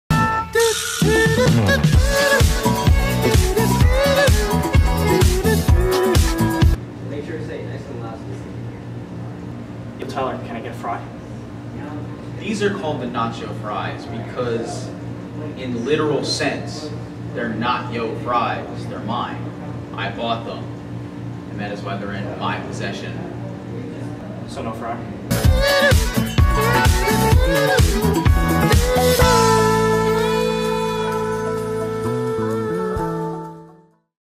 NOT-YO-FRIES
This video is a parody of Report of the weeks nacho fries bit that i made in my video class. hope you enjoyed